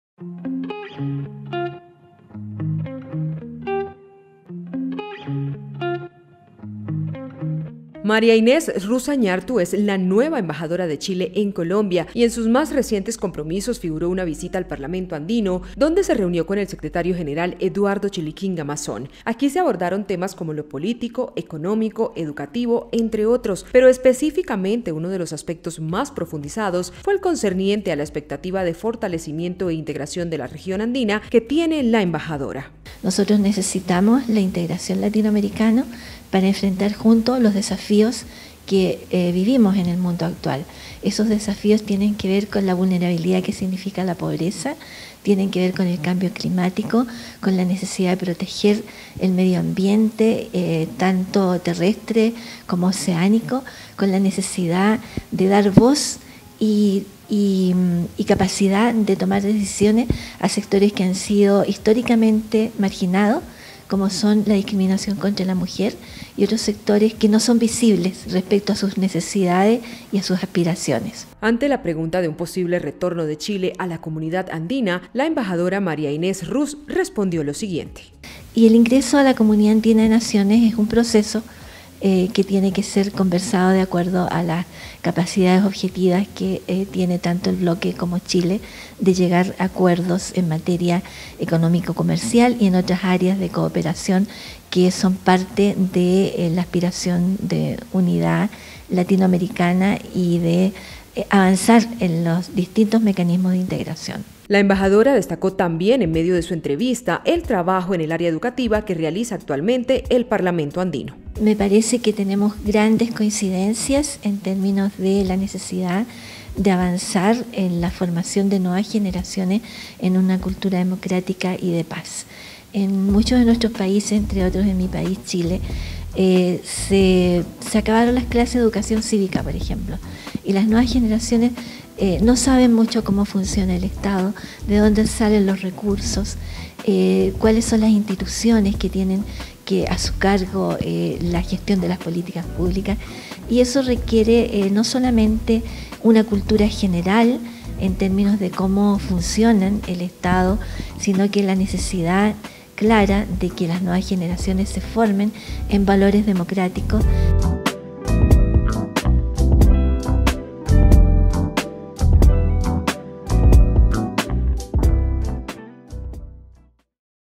Embajadora de Chile se reúne con secretario general del Parlamento Andino
María Inés Ruz Zañartu, quien es la nueva Embajadora de Chile, aseguró en una reunión sostenida con el secretario general del Parlamento Andino, que los esfuerzos por seguir fortaleciendo la integración de la región Andina, así como el trabajo realizado con el organismo; siguen estando como prioridad en sus expectativas de trabajo. En la conversación se habló a su vez de economía, educación, entre otros detalles.